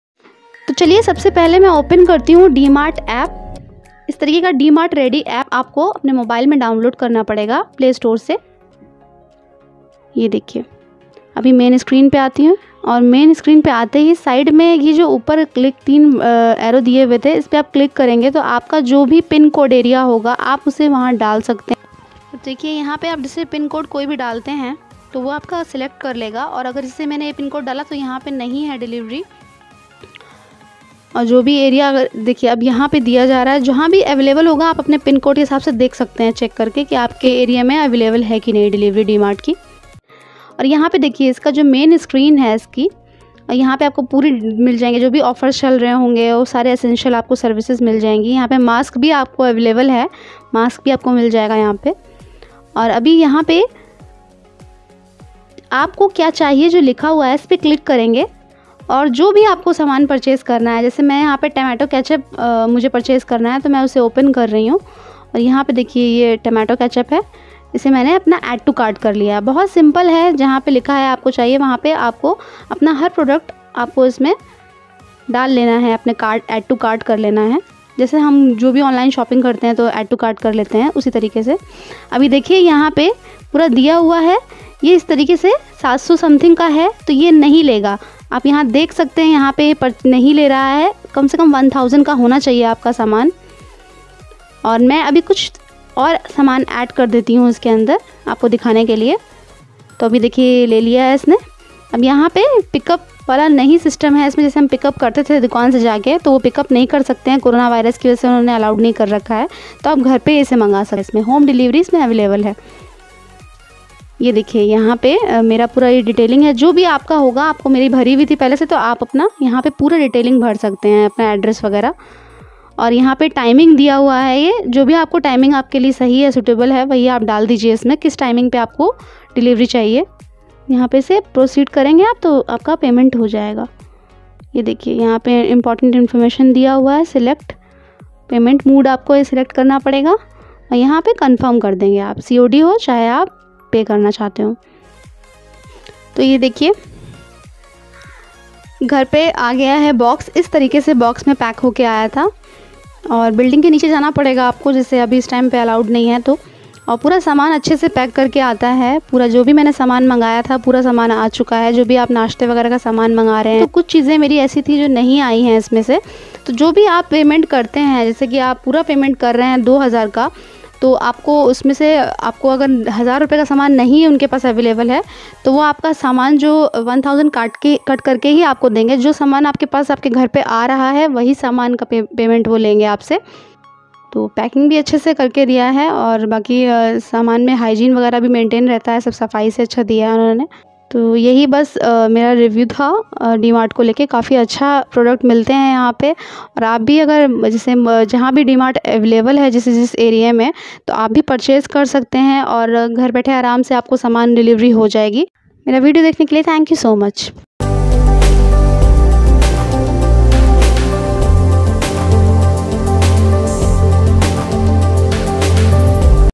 DMart Home Delivery | डी मार्ट App से order बुक करके घर पे मंगवाएं
DMart Ready App Home Delivery | डी मार्ट App से order बुक करके घर पे मंगवाएं
How to order from DMart Ready App
DMart Ready, It is very good app to get more discount and get grocery at home.

Thanks
Manira